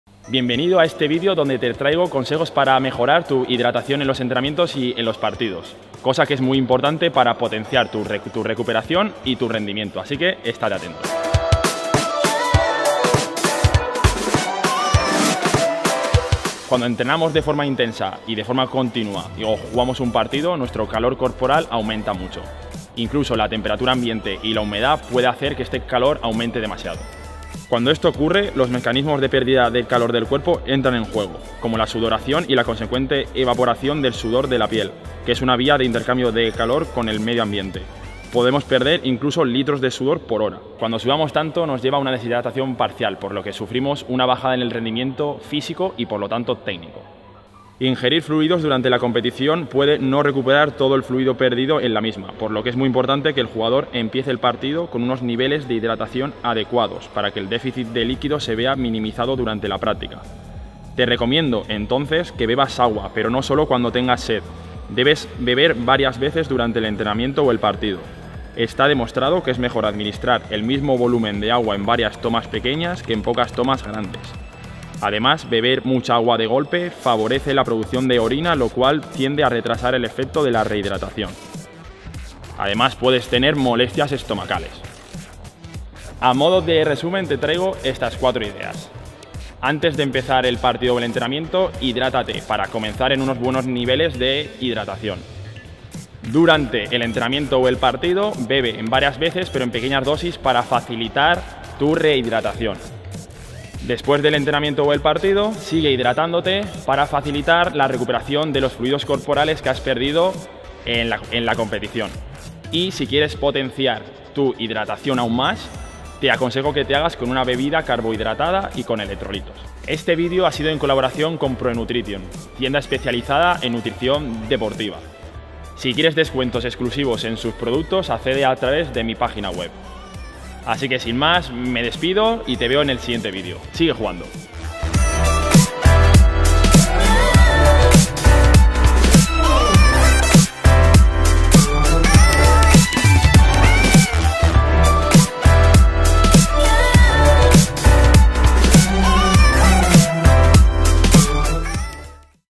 Mejora tu Hidratación en partidos y entrenamientos
Consejos para mejorar tu hidratación en los partidos y entrenamientos, en colaboración con Proenutrition.com, tienda especializada en nutrición deportiva. Si quieres saber más sobre cómo recuperarte mejor tras un partido: visita mi página web